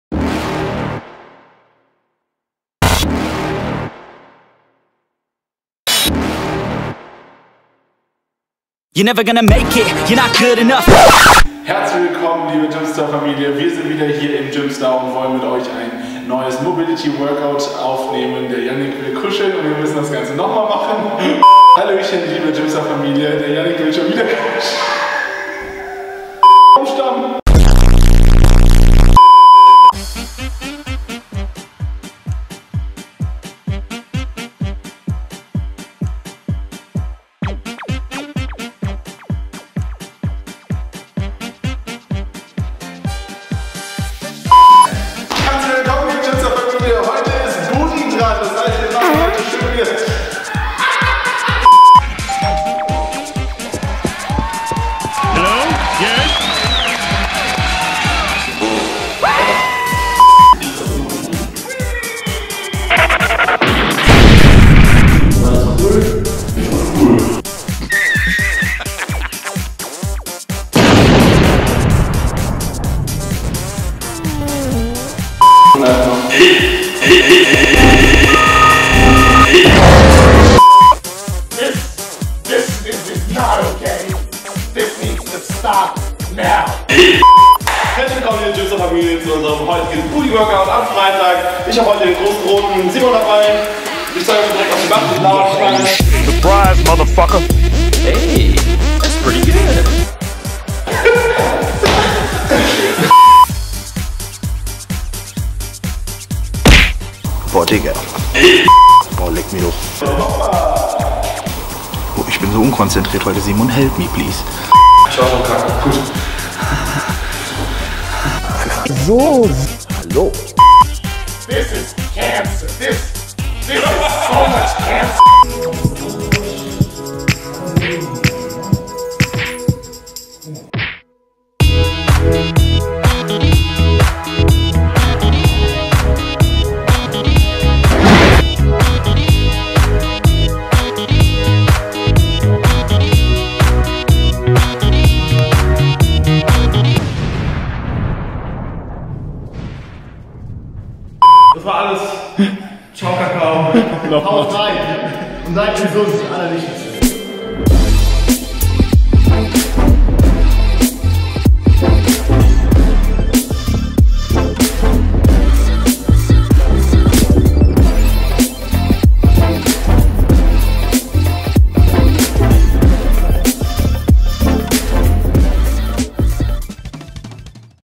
GYMSTAR_Outtakes_Q1_2021.exe
YES!
Ein Quartal ist wieder rum und wir bereichern euch mit unserer neusten Outtake Compilation im .exe Format!
Wir hoffen mit dem Video euren derzeitigen Alltag etwas Spaß und Freude hinein zu bringen und das ein oder andere Lächeln oder gar Lachen hervorzubringen.
VIEL SPAß - EURE GYMSTAR CREW